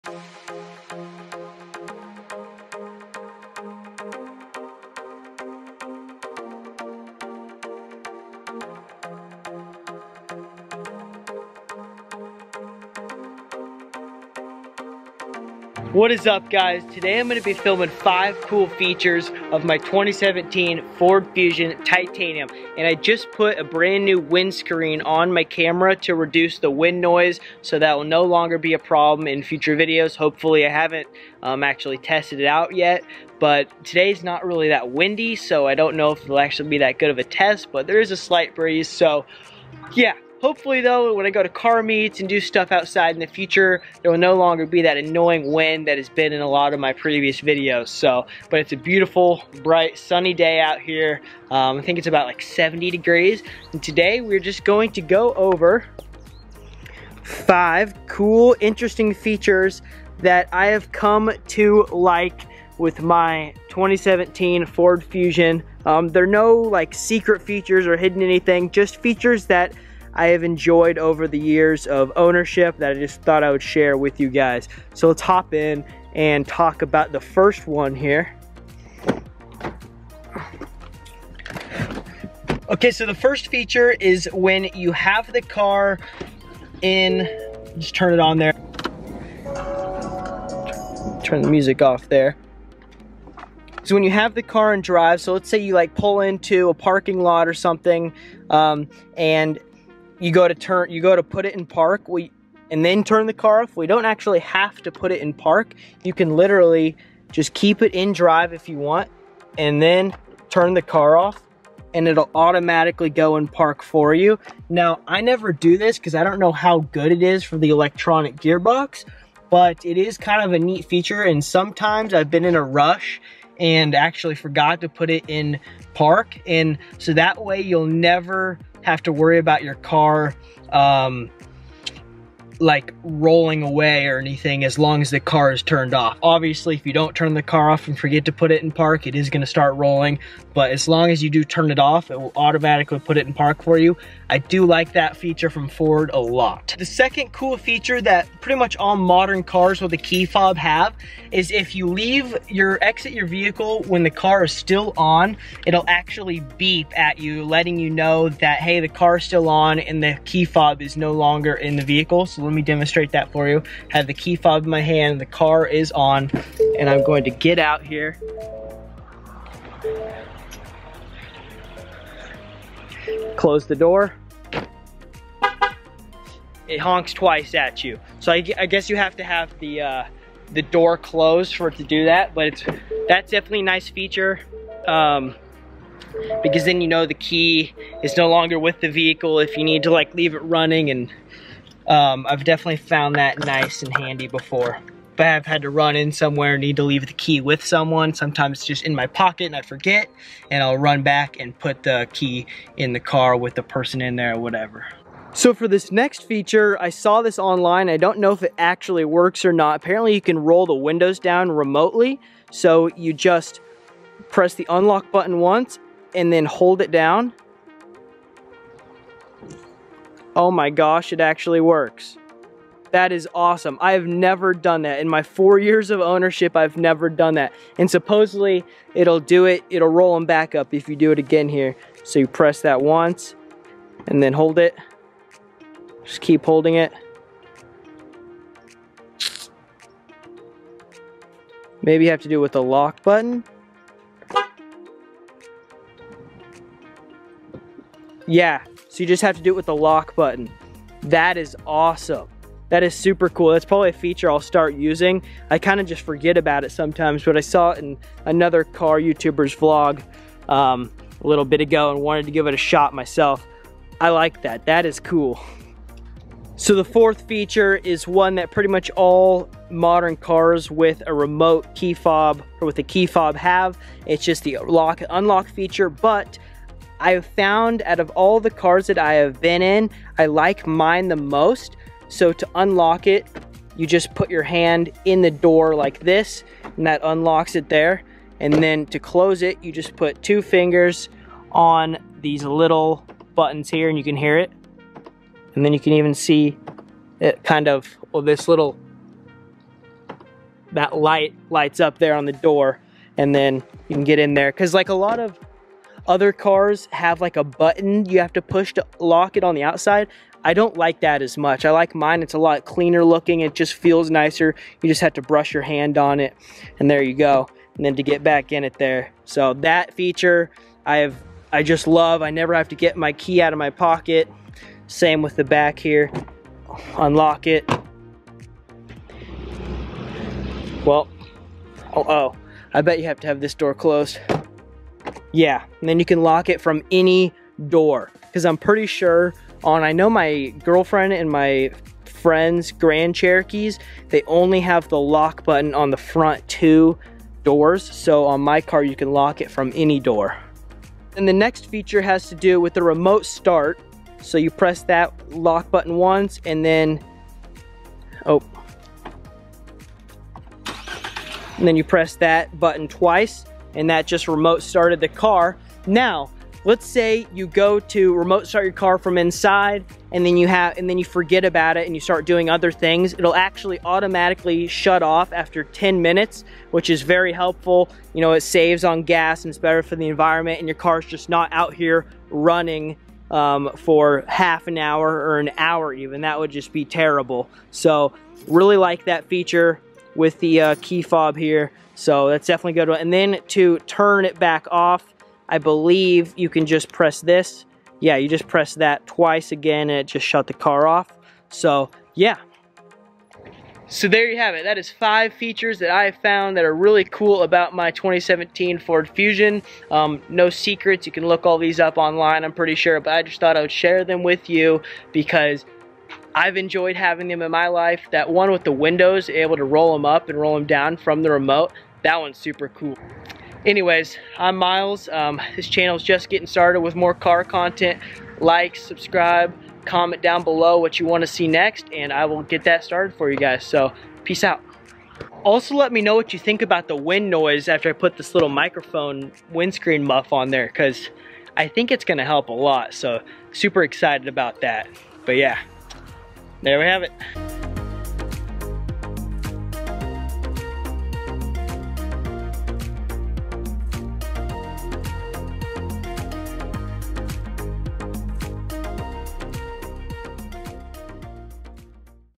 5 INSANE FEATURES OF MY FORD FUSION
In this video I go over five cool and strange features of my 2017 Ford Fusion that you may or may not know about.

milesobishop